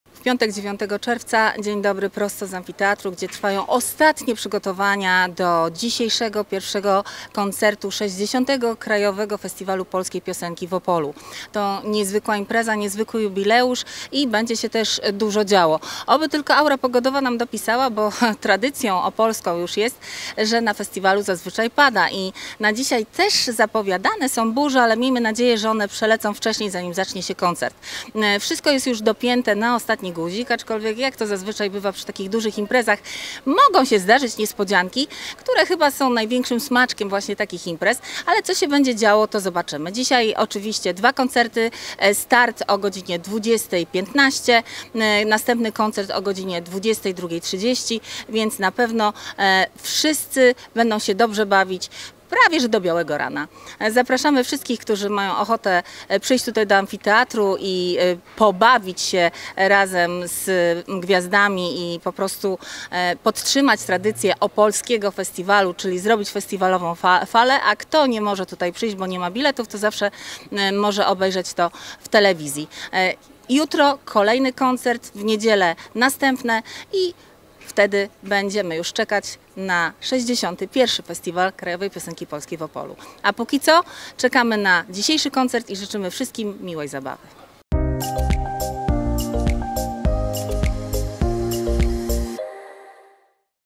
Dzień dobry tu Opole (piątek, 9 czerwca 2023r.)
Ostatnie szlify w opolskim amfiteatrze i już dzisiaj o 20:15 pierwszy koncert na 60 KFPP.